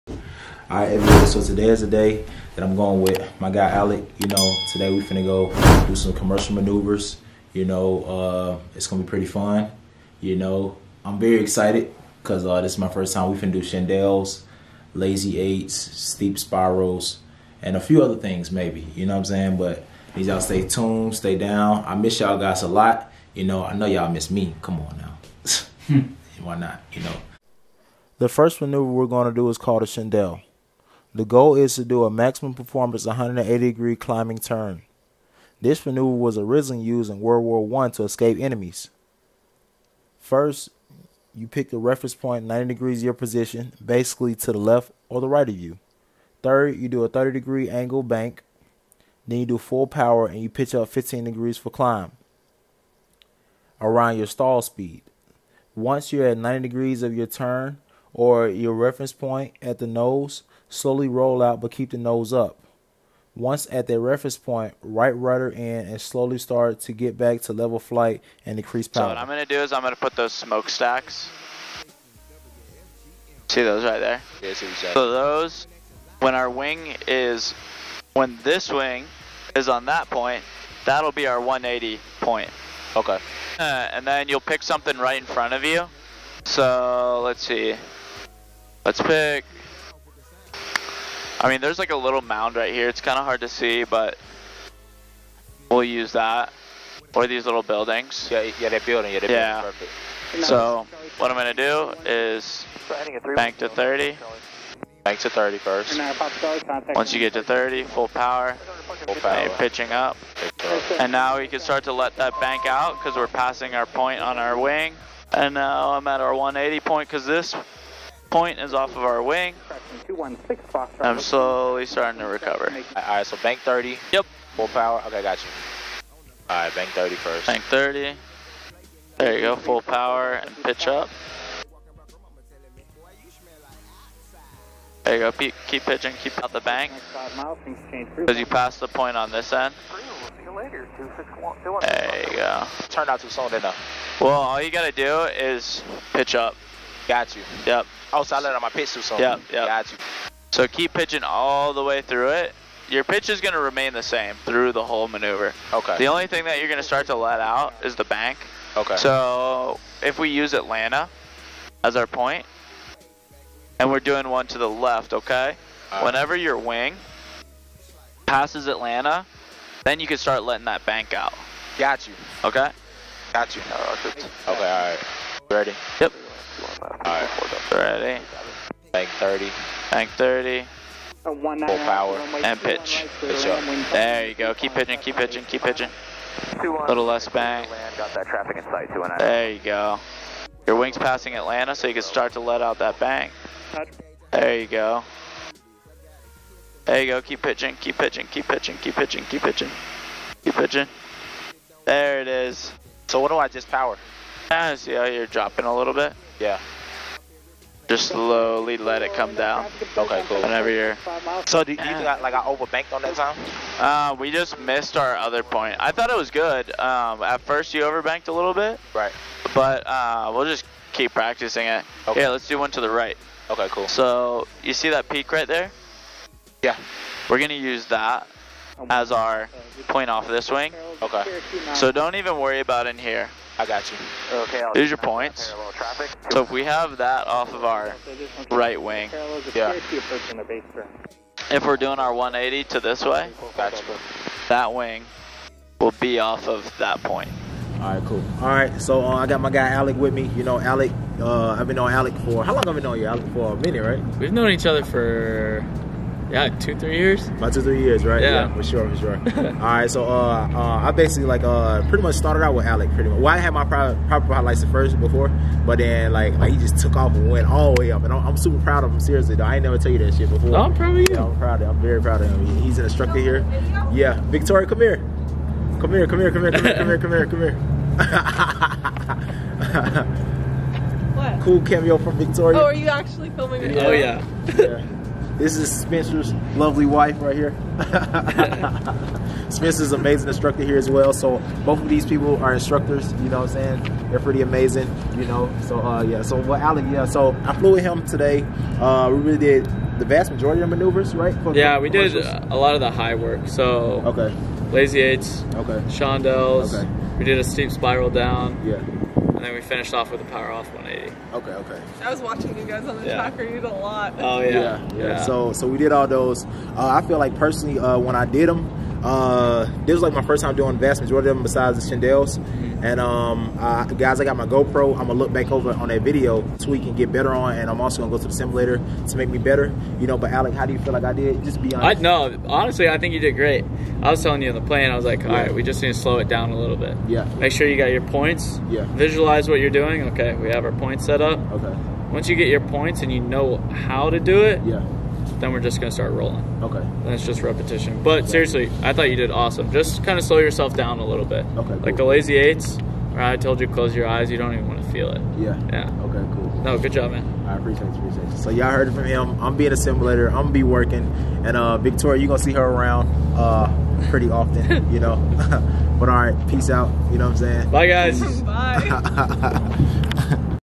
chandelle commercial maneuver
I will take you through the necessary steps to complete the CHANDELLE maneuver at the commercial pilot level in a piper cherokee.